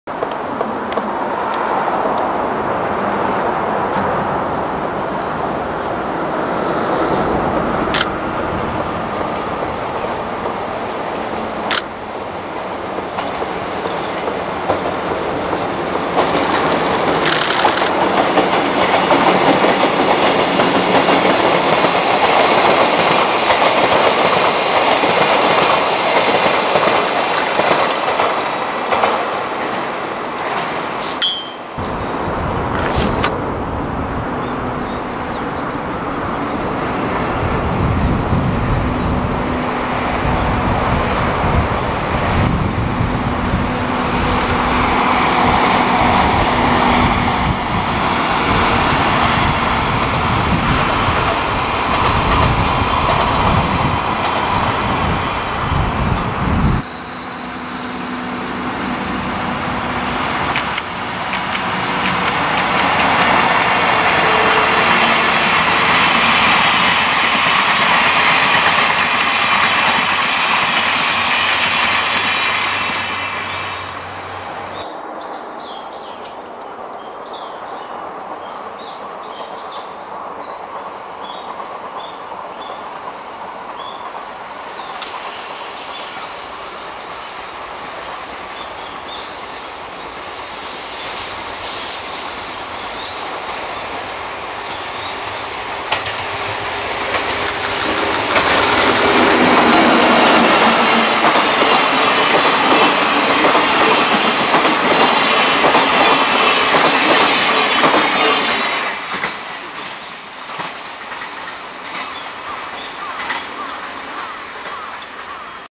【青い12系号・みなかみ物語号】送り込みor返却回送 2015/11/13.16
磐越西線で使用する高崎の12系送り込みと、上越線で使用する新津の12系の送り込みor返却回送を撮影。